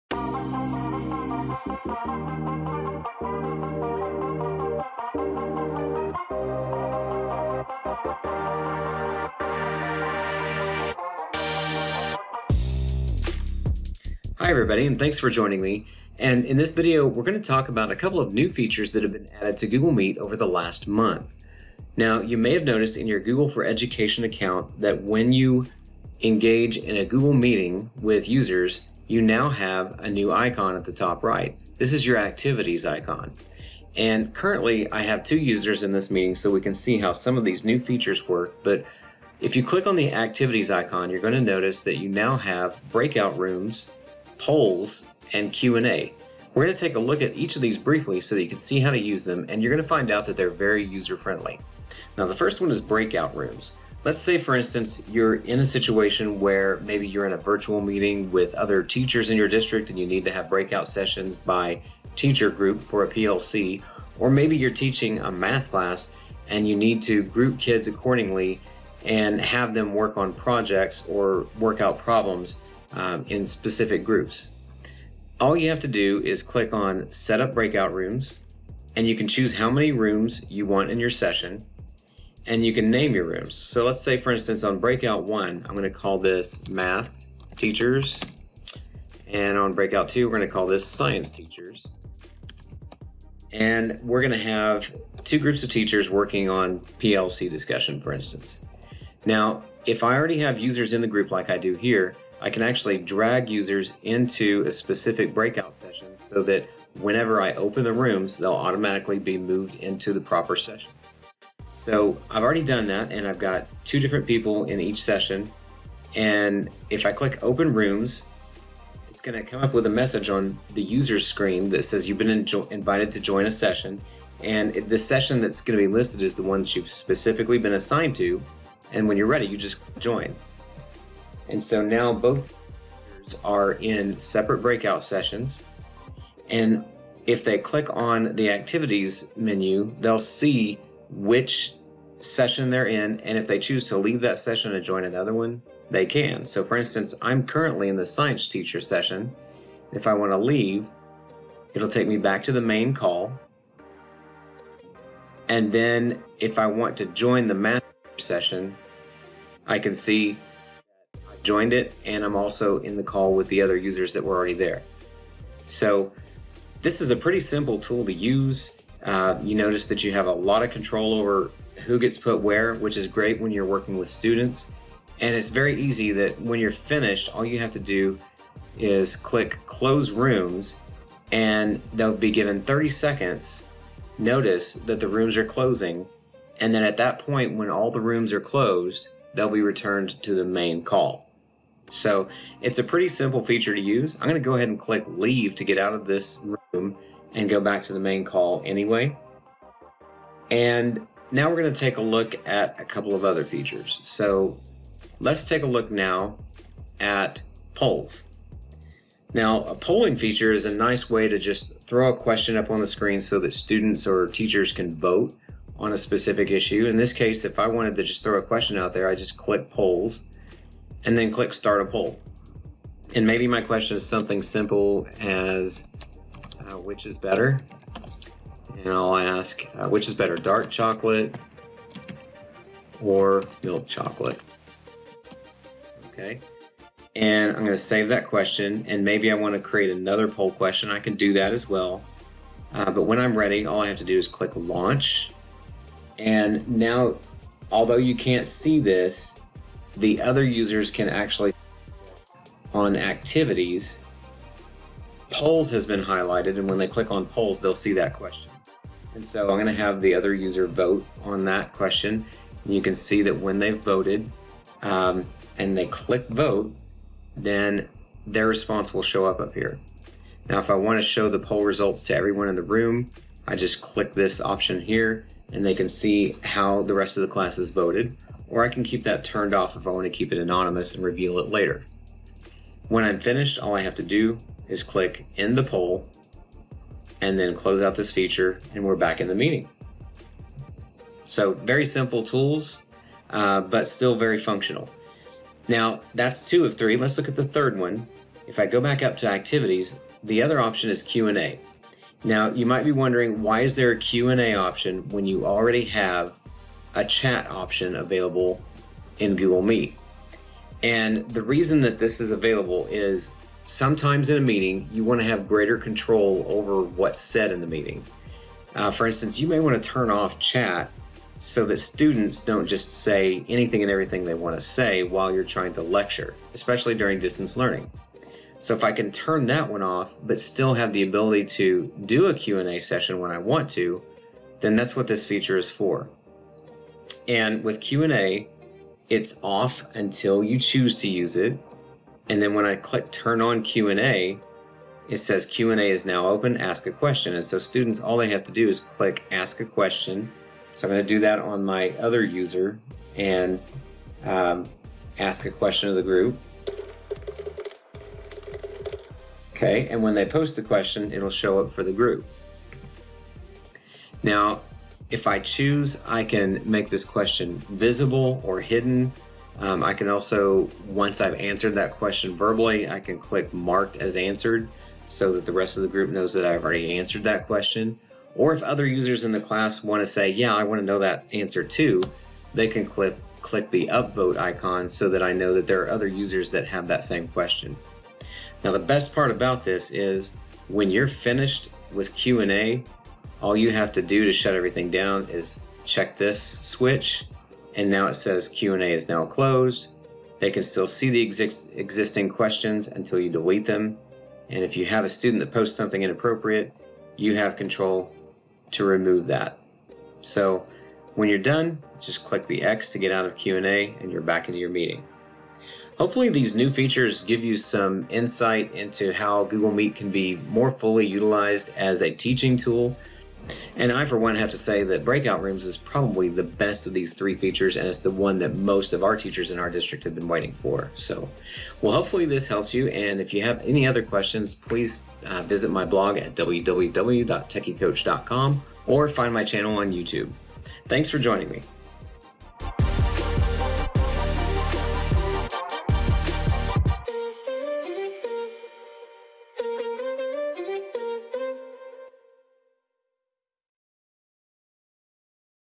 3 Must Have New Features of Google Meet
This video features an explanation of 3 new tools inside Google Meet:  Breakout Rooms, Q&A and Polls